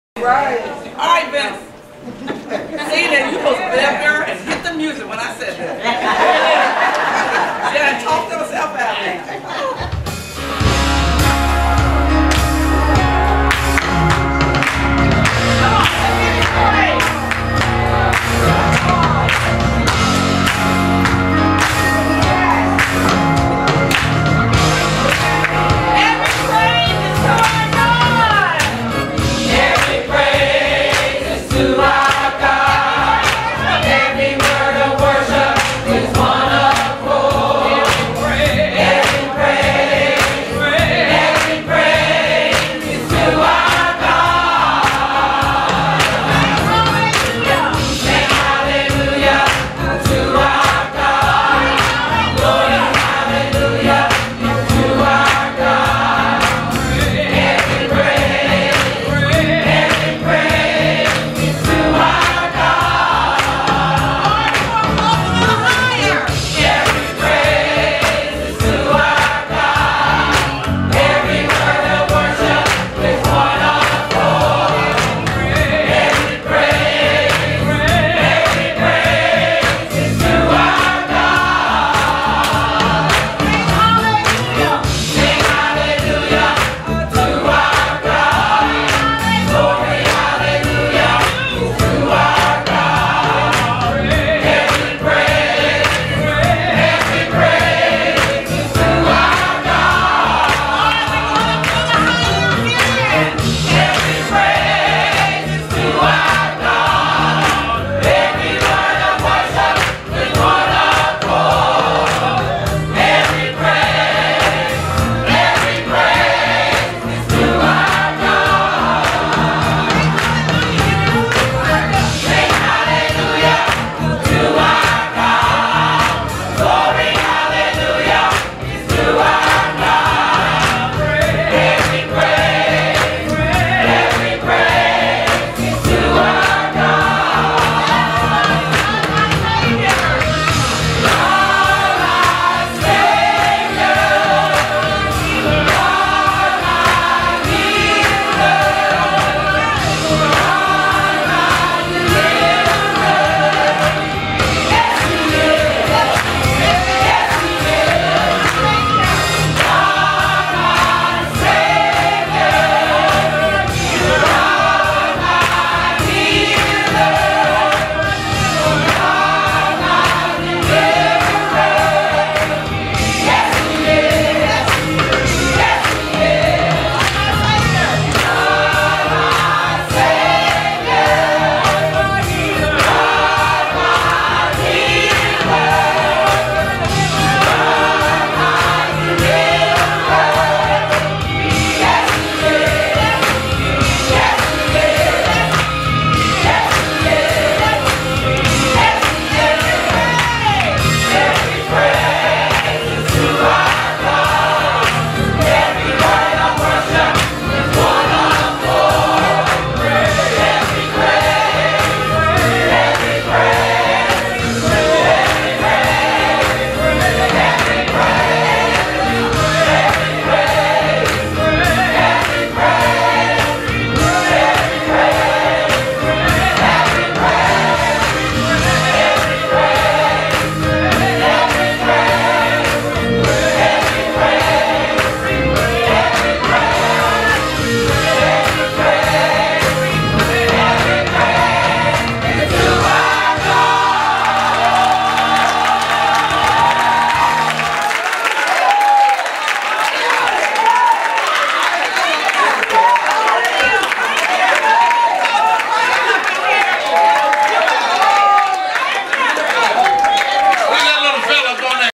Wainwright Mass Choir
Wainwright Mass Choir at the Page Jackson Alumni Gospel Fext